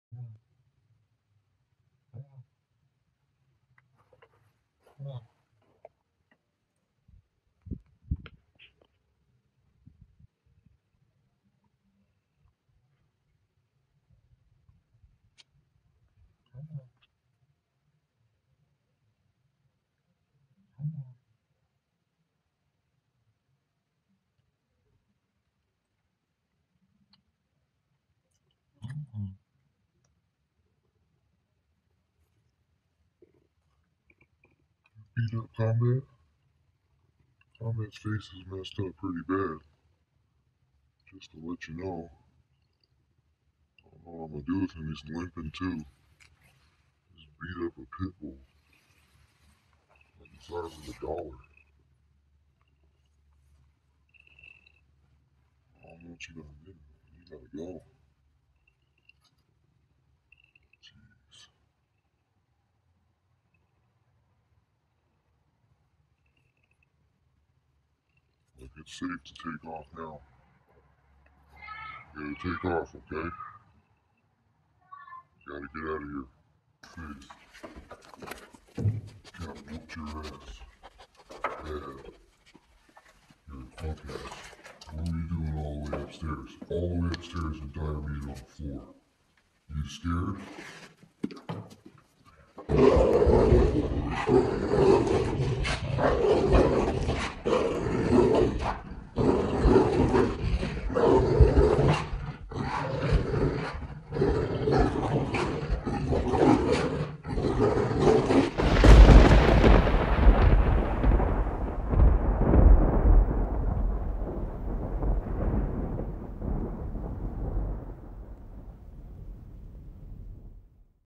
cat whooped my pit bull
Black cats are dangerous and should be banned, how many more pit bulls need to be violated before this happens?  My shirts